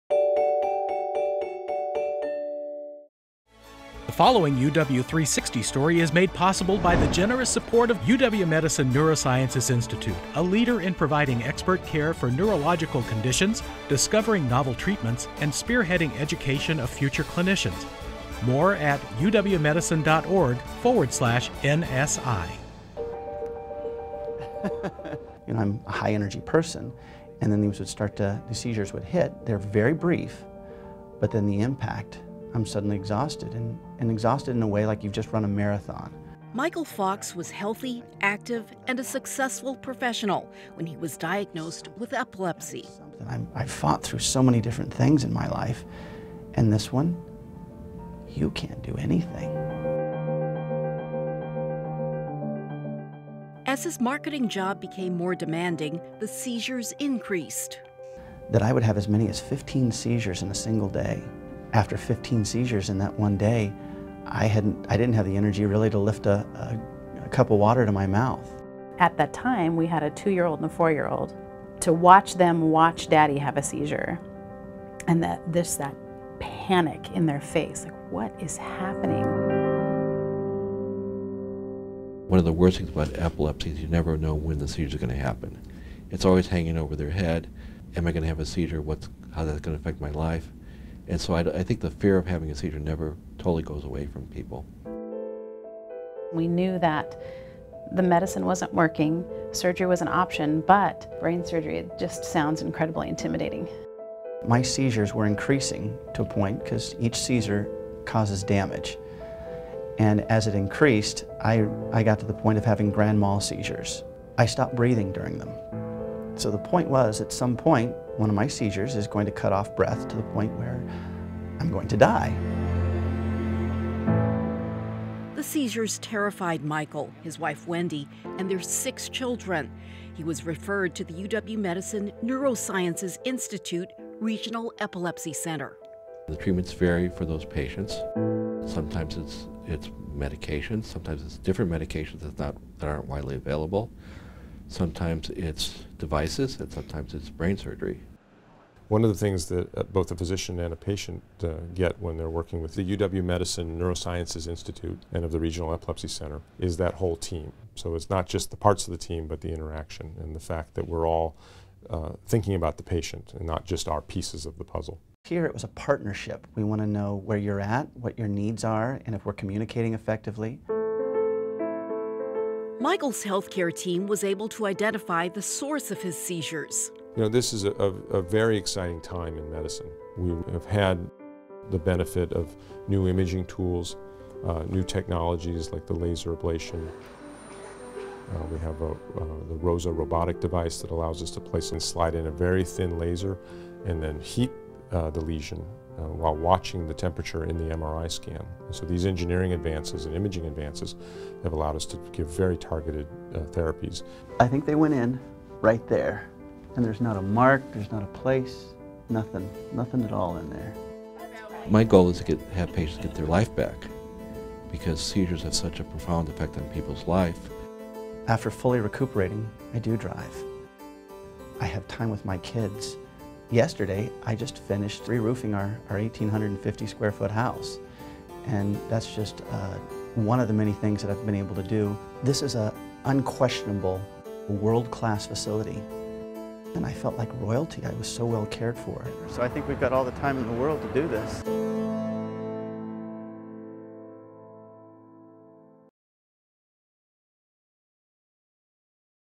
Treating Epilepsy
Epilepsy affects about 3 million people in the U.S. Some have occasional seizures, while others can have them every day. But now, some UW Medicine neurological patients have found relief from their seizures, thanks to a high-tech piece of equipment called the "Rosa Robot."

Michael Fox, patient, Regional Epilepsy Center, UW Medicine
Wendy Fox, spouse of Michael Fox
John W. Miller MD, PhD, director, Regional Epilepsy Center, UW Medicine
Jeffrey G. Ojemann, MD, professor, Neurological Surgery, UW Medicine

10/04/17